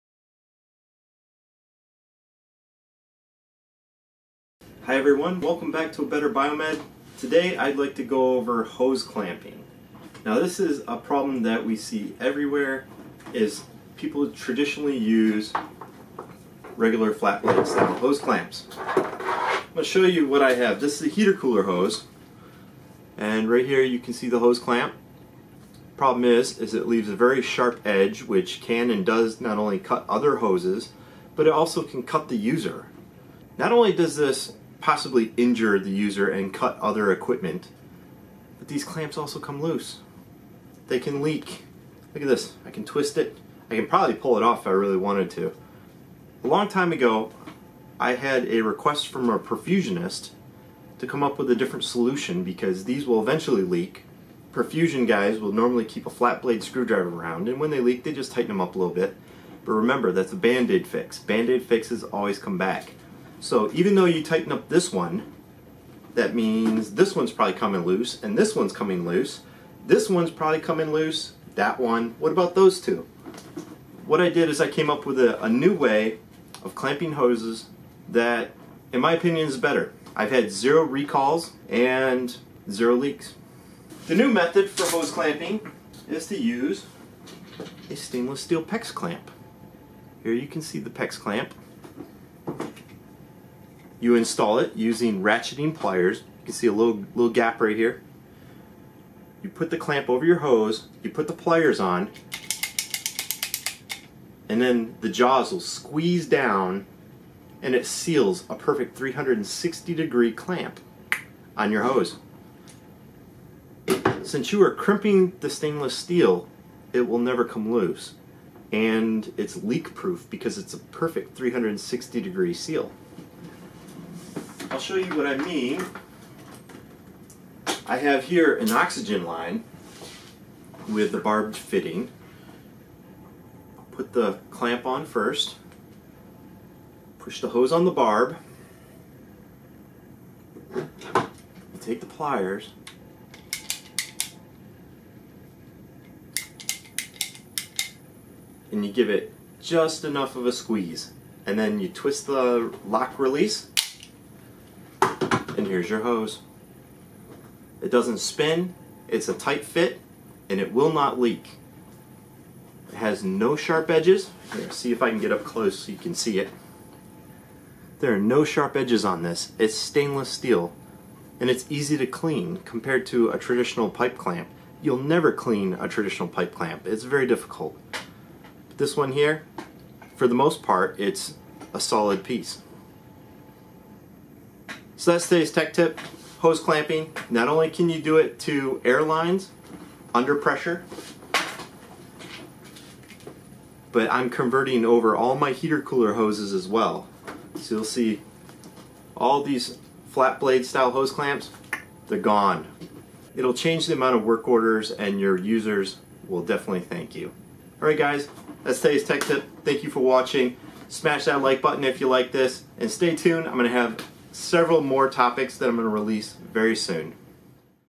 Pro Tip 3: Hose Clamping
Permanent fix to leaky and loose hoses. Minimal user error compared to ferrules or screw clamps.